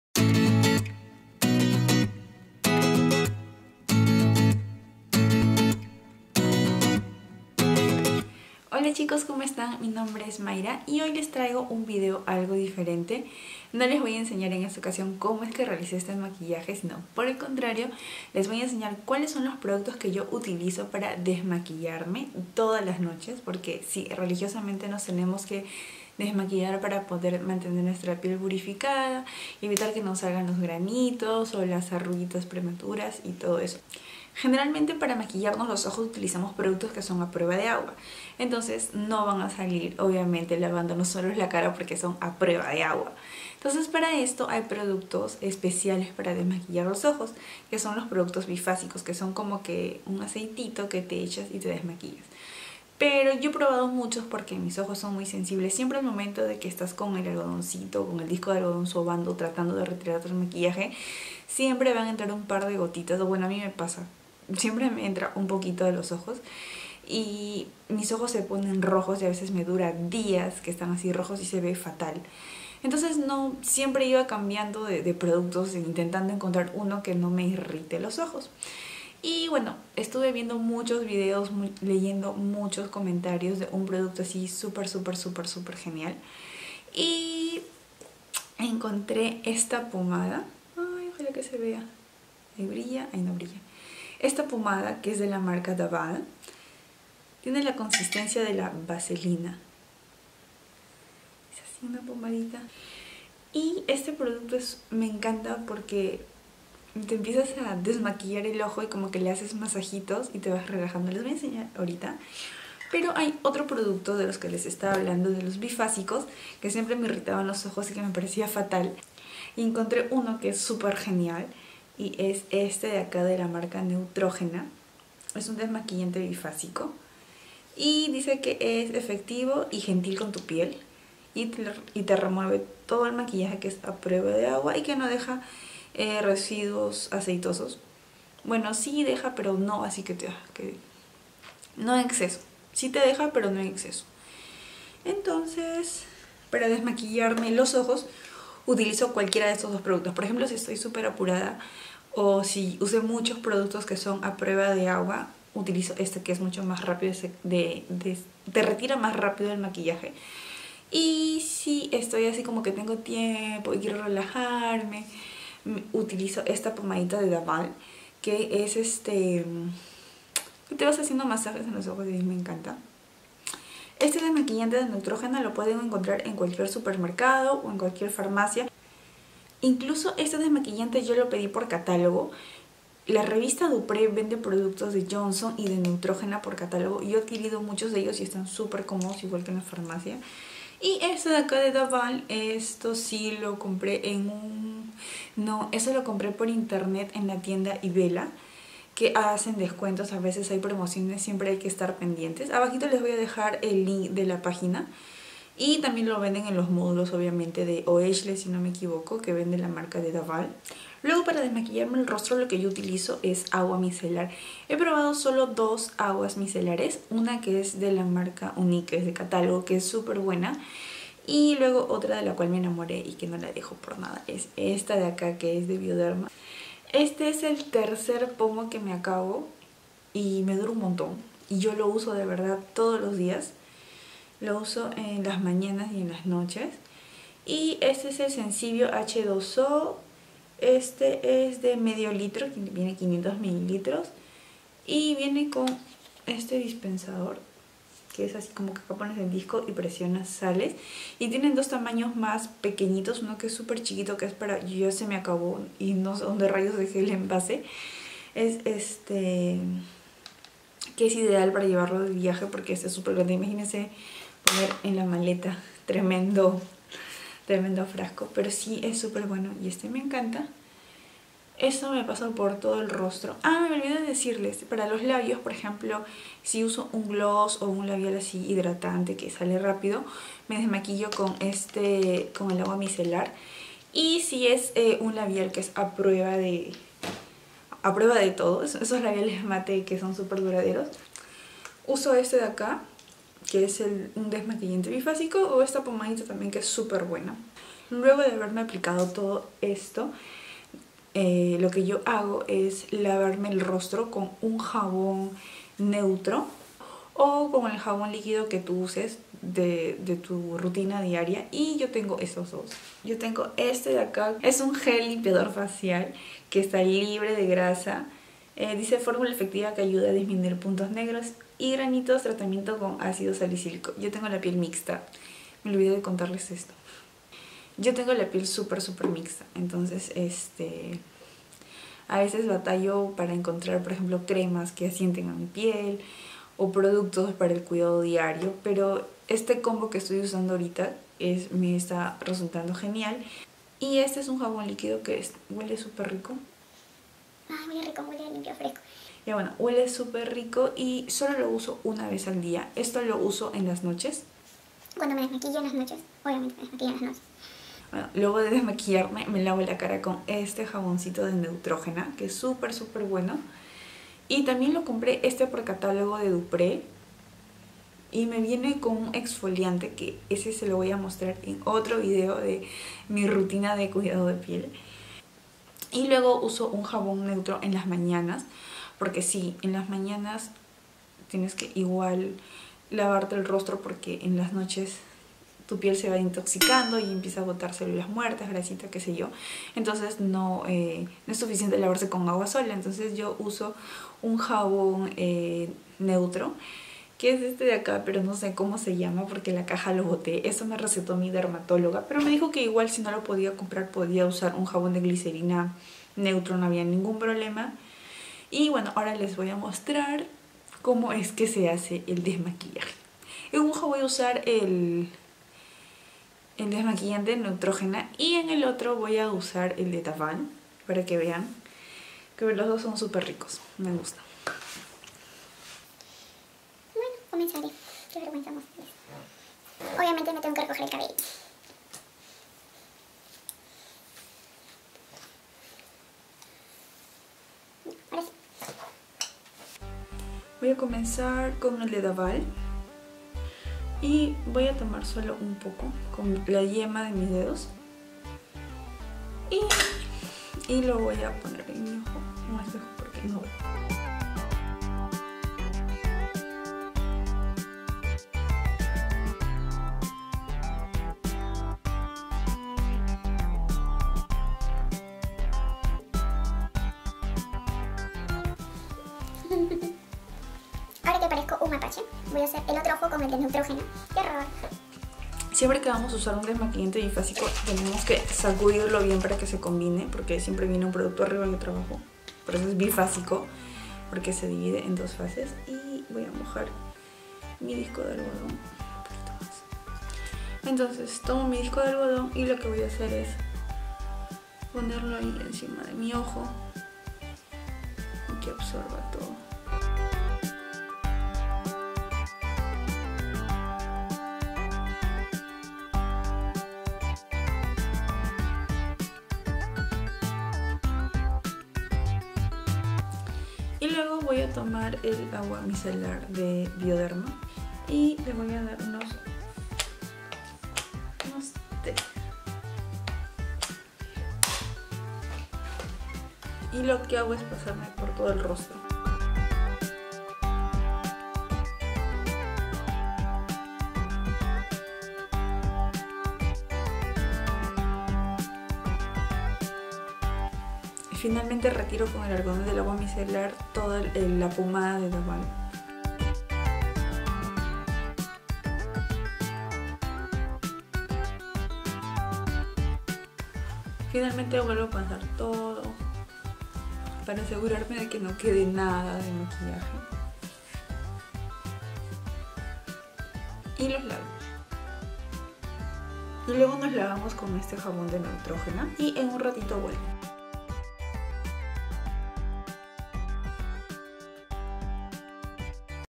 ¿Cómo me desmaquillo? / Mi rutina de Noche 🌙
Hola chic@s
Aquí les dejo el link de la tienda Ibella, en donde compre el agua micelar de Bioderma y el Balms Away de The Balm:

🎵Musica de Youtube 🎶